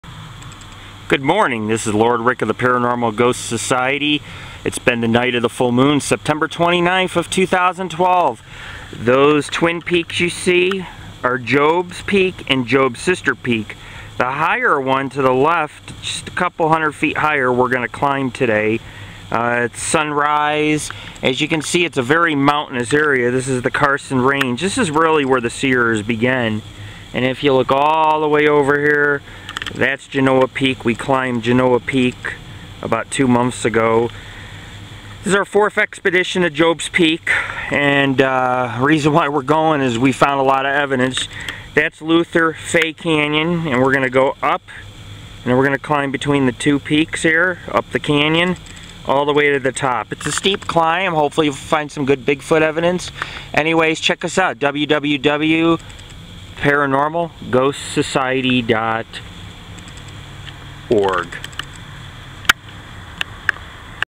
Jobs Peak Part 51 - "Goodmorning Welcome To Expedition Four"
We started off expedition four at sunrise our goal is to climb up into Luther Canyon then eventually between Jobs and Jobs Sister peak till we reach the summit. From afar this may not look like much but it is truly one of the highest peaks within the Carson Range. From a distance it just looks as if this is a large granite mountain up close trees and rock formation grace this monster.